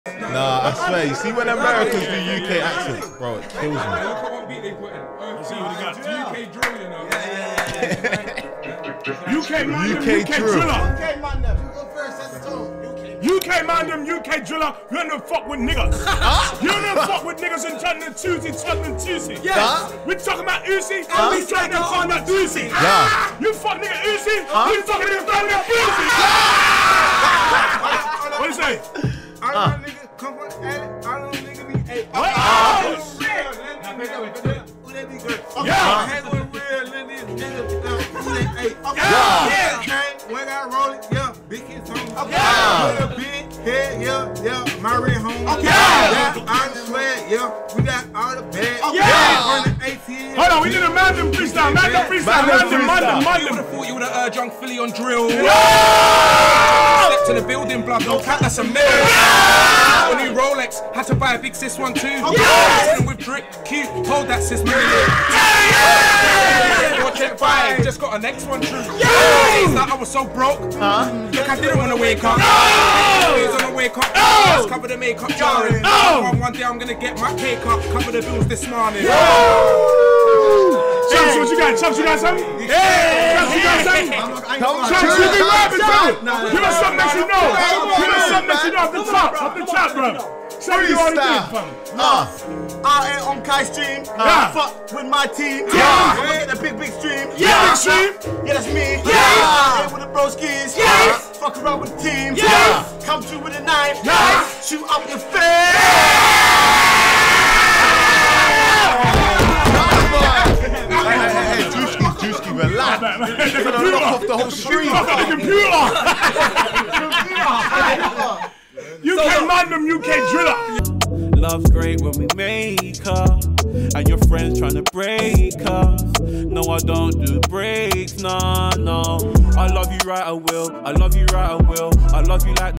Chunkz and Filly Freestyle on Kai Cenat’s Stream!
In this video, Samham reacts to Kai Cenat x Druski Live Stream